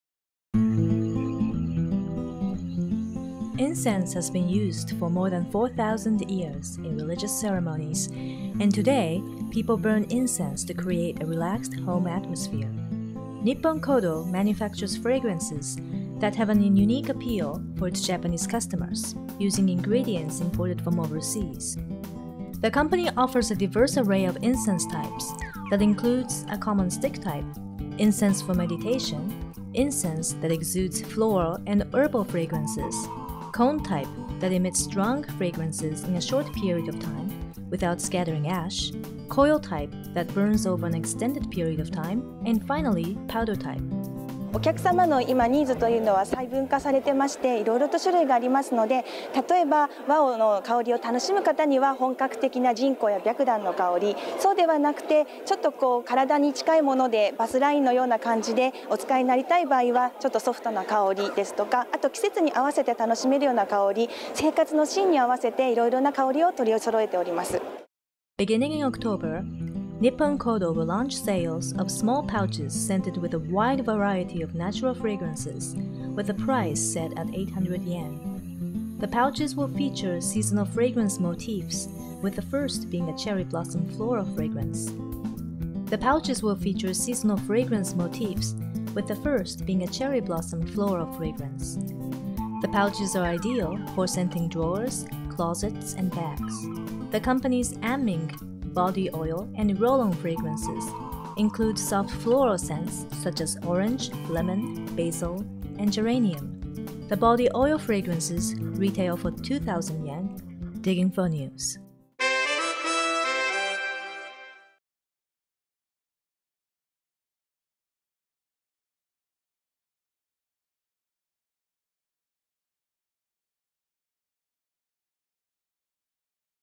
Japanese Fragrances : DigInfo
Incense has been used for more than 4,000 years in religious ceremonies, and today people burn incense to create a relaxed home atmosphere.

Nippon Kodo manufactures fragrances that have an unique appeal for its Japanese customers using ingredients imported from overseas.  The company offers a diverse array of incense types that includes a common stick type, incense for meditation, incense that exudes floral and herbal fragrances, a cone type that emits strong fragrances in a short period of time without scattering ash, coil type that burns over an extended period of time, and finally a powder-type.

Beginning in October, Nippon Kodo will launch sales of small pouches scented with a wide variety of natural fragrances with the price set at 800 yen.  The pouches will feature seasonal fragrance motifs with the first being a cherry blossom floral fragrance.  The pouches are ideal for scenting drawers, closets, and bags.

The company's "Anming" body oil and roll-on fragrances include soft floral scents such as orange, lemon, basil, and geranium.

The body oil fragrances retail for 2,000 yen, while small, convenient roll-on fragrance dispensers retail for 1,200 yen.